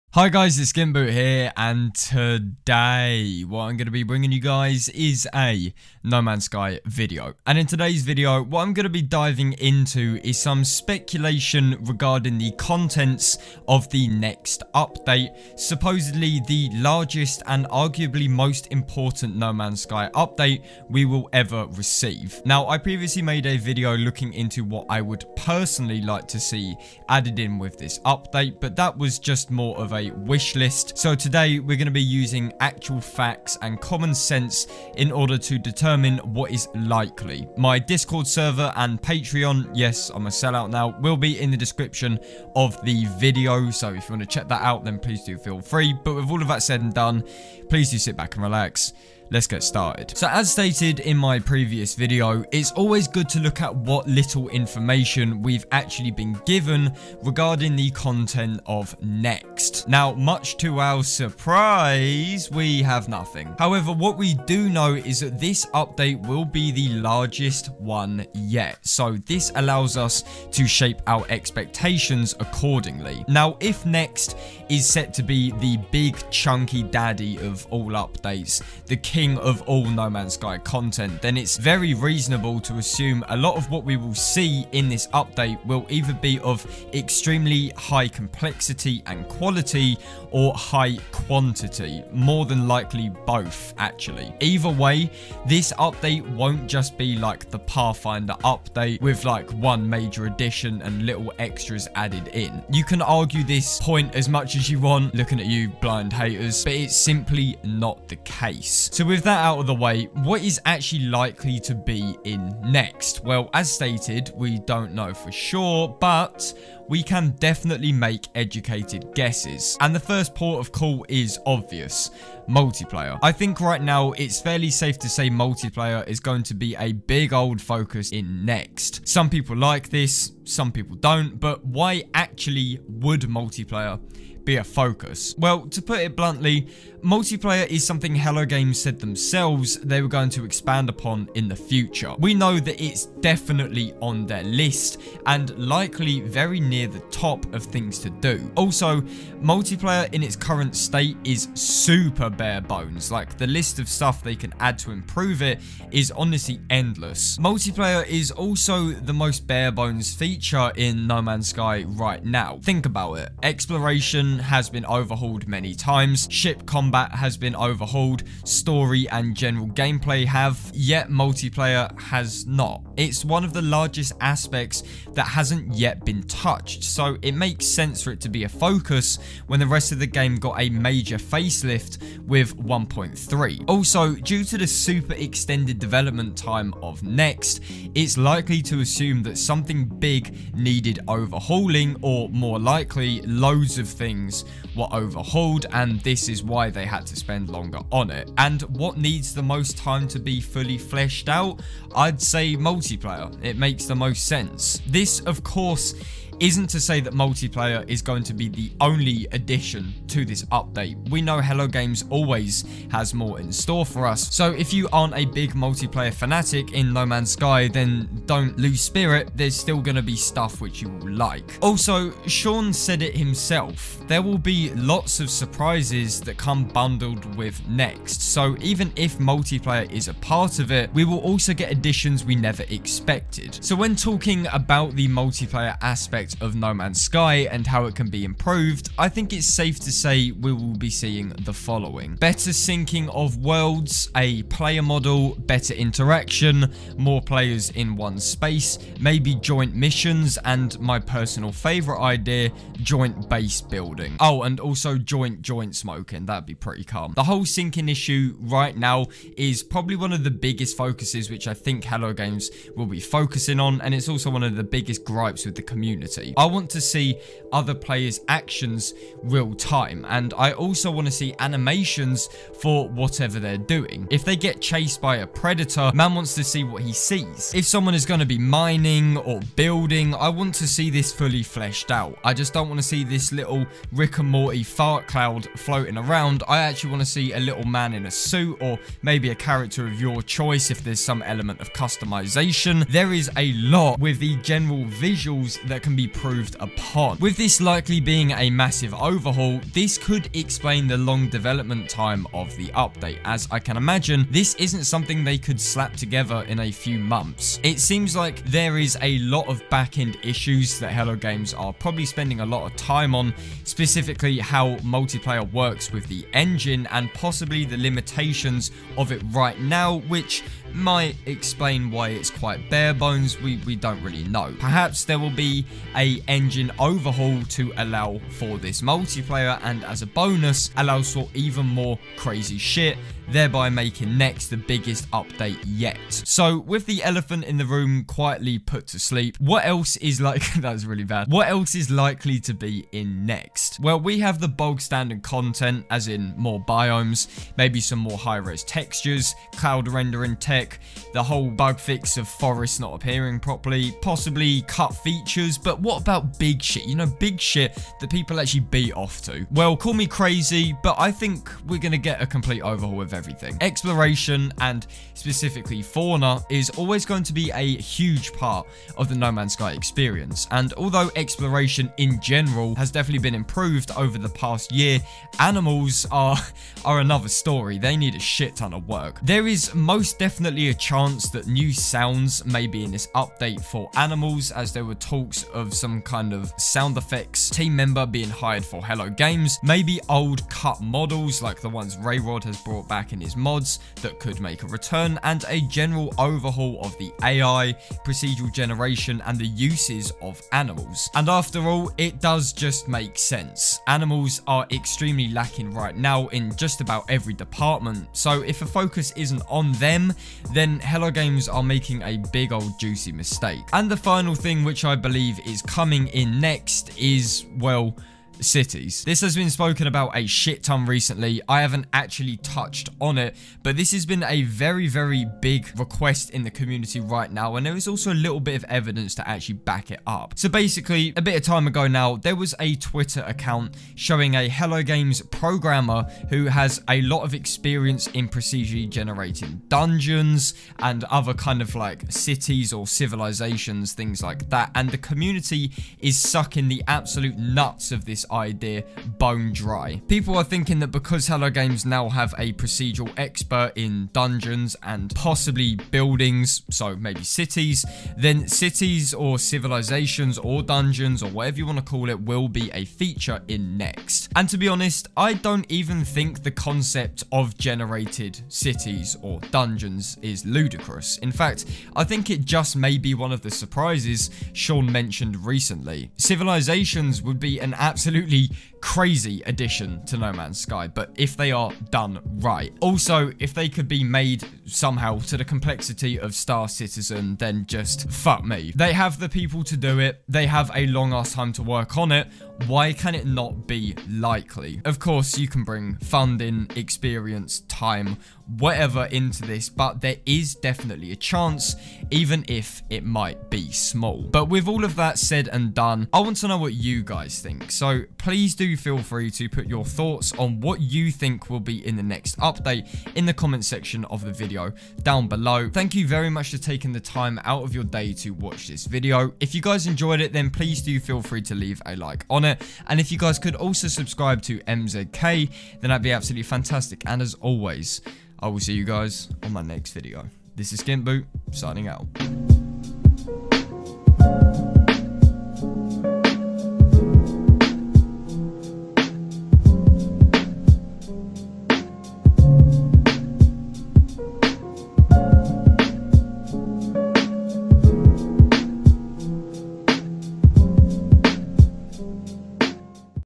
No Man's Sky - What Is Likely To Be In The "NEXT" Update?
Hello everyone! Today I'm back with a brand new video looking into what we're likely to see added into No Man's Sky through the upcoming "NEXT" update.

Add me on Steam - "SkintBoot"